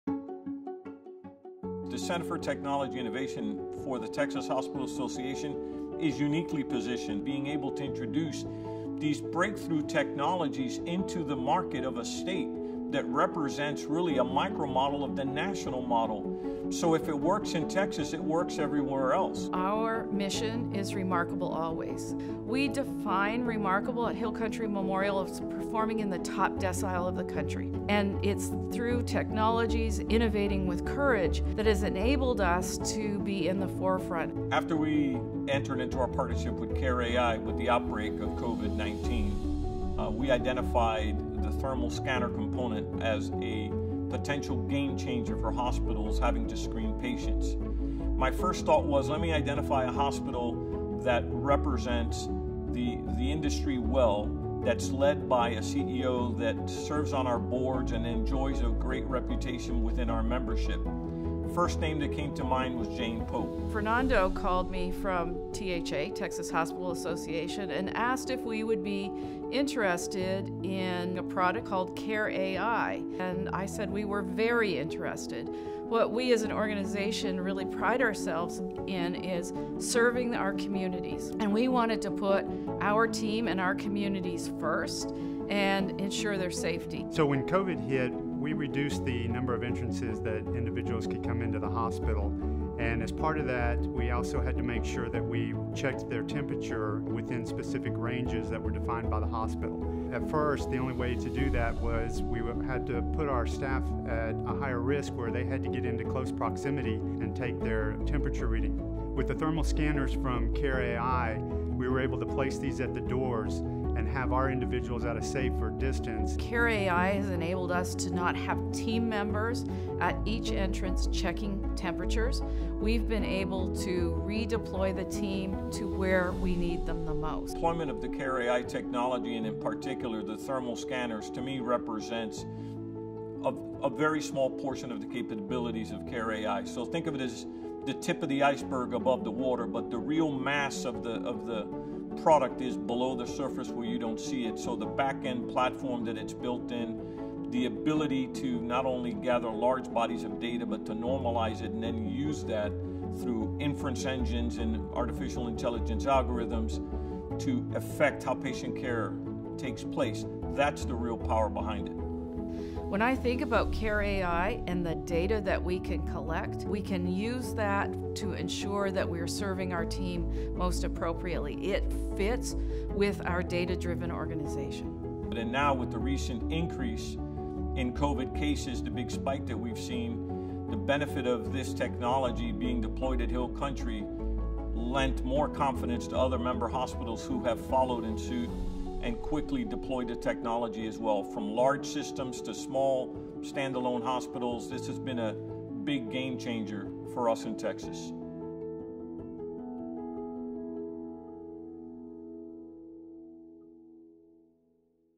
Texas Hospital Association & Hill Country Memorial - care.ai™ Case Study
We partnered with the Texas Hospital Association to accelerate the adoption of AI-powered AutonomousMonitoring across Texas. Hill Country Memorial Hospital deployed our platform during the COVID19 crisis to build a safeworkplace.